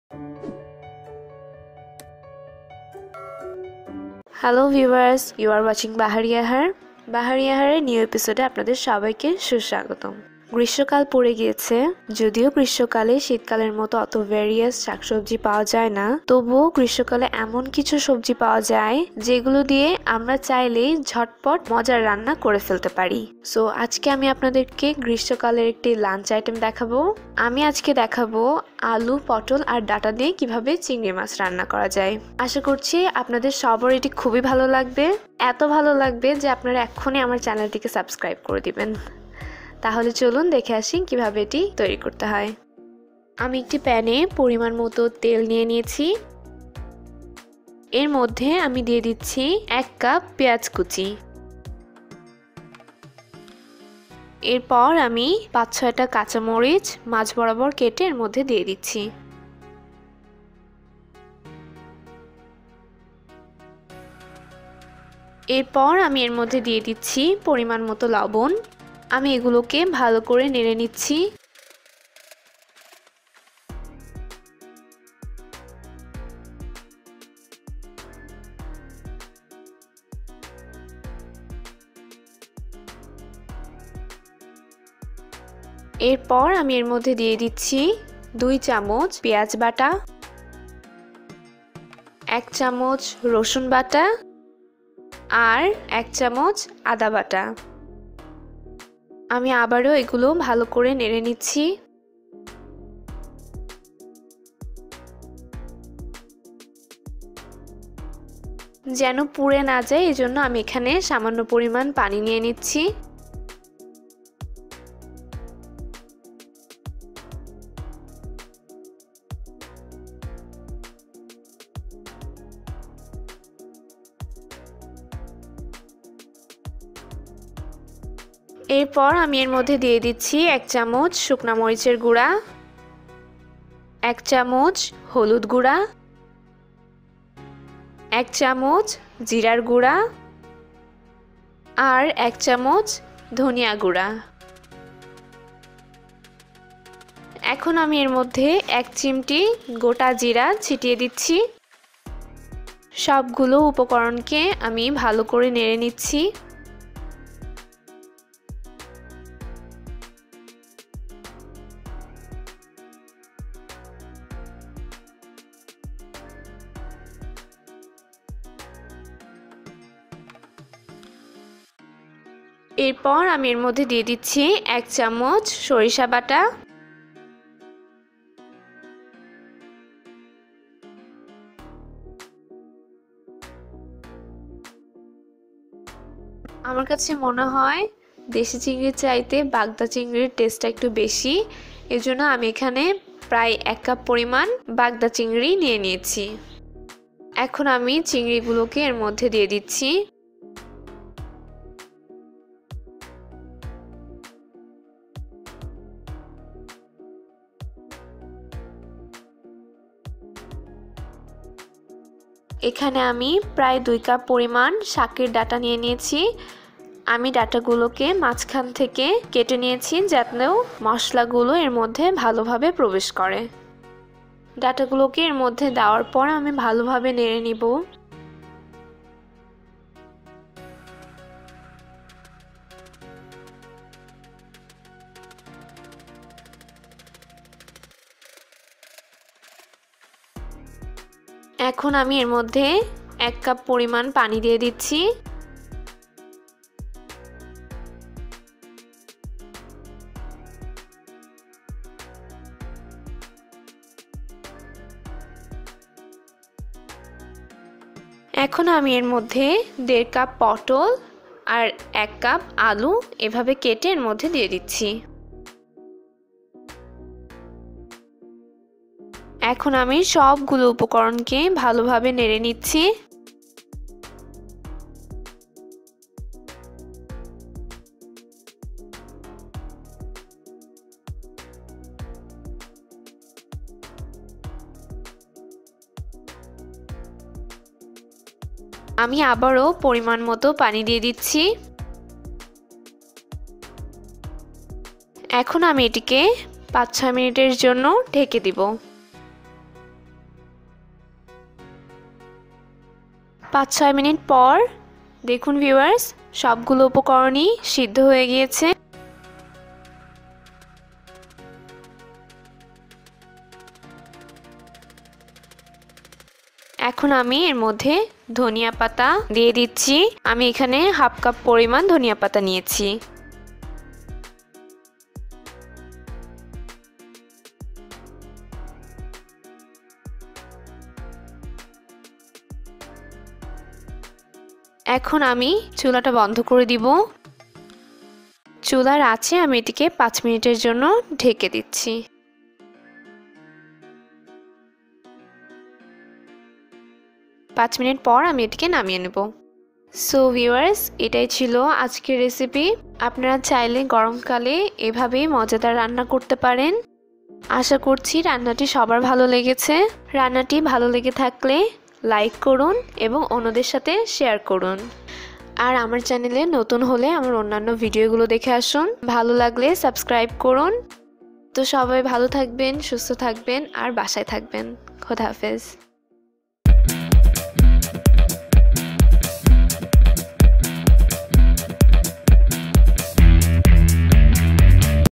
চেটেপুটে খাওয়ার মত অতুলনীয় স্বাদের ডাটা চিংড়ি রেসিপি | Data with Shrimp | Data cingri recipe
এখন ডাটা শাকের মৌসুম। এটি অনেকেরই প্রিয় একটি সবজি। স্বাস্থ সুরক্ষাই এই সবজিটি গুরুত্বপূর্ণ ভূমিকা রাখে। ডাটা শাকে রয়েছে প্রচুর পরিমাণে ভিটামিন সি Vitamin C , ফলিক এসিড যা ত্বকের জন্য খুবই উপকারী। এই ভিটামিনগুলো প্রতিদিনের পুষ্টি জোগায়, ত্বক, চুলের ক্ষয় রোধ করে এবং রোগ প্রতিরোধ ক্ষমতা বৃদ্ধি করে। আবার মজাদার চিংড়িরও কিন্তু অনেক পুষ্টিগুণ রয়েছে। চিংড়িতে রয়েছে প্রায় ১৪ শতাংশ ফসফরাস। তাই চিংড়ি খেলে হাড়ের ক্ষয় রোধ হওয়ার পাশাপাশি হাড় হয় মজবুত ও শক্ত করবে। চিংড়িতে থাকা ম্যাগনেশিয়াম ডায়বেটিসের হাত থেকে রক্ষা করে। আজ বাহারি আহার Bahari Aahr এ দেখাবো চেটেপুটে খাওয়ার মত অতুলনীয় স্বাদের ডাটা চিংড়ি রেসিপি Data with Shrimp । যা দেহের রোগ প্রতিরোধ ক্ষমত বৃদ্ধ করবে। ডাটা দিয়ে চিংড়ি রান্নার রেসিপি একবার খেলে বার বার ক্ষেতে ইচ্ছা করবে। খুব সহজেই ছোট চিংড়ি মাছ দিয়ে চিকন কাটোয়ার ডাটা Data cingri recipe রেডি করে ফেলতে পারেন। চলুন দেখে আসি নানা পুষ্টি গুনে ভরা Chingri mach diye Data Recipe.

উপকরনঃ
তেল
লবণ
পেয়াজ বাটা ২ চামচ
রসুন বাটা ১ চামচ
আদা বাটা ১ চামচ
শুকনা মরিচ গুড়া ১ চামচ
হলুদ গুড়া ১ চামচ
জিরার গুড়া ১ চামচ
ধনিয়া গুড়া ১ চামচ
গোটা জিরা ১ চিমটি
সরিষা বাটা ১ চামচ
বাগদা চিংড়ি ১ কাপ
শাকের ডাটা ২ কাপ
পটল ১/২ কাপ
আলু ১ কাপ
ধনিয়া পাতা ১/২ কাপ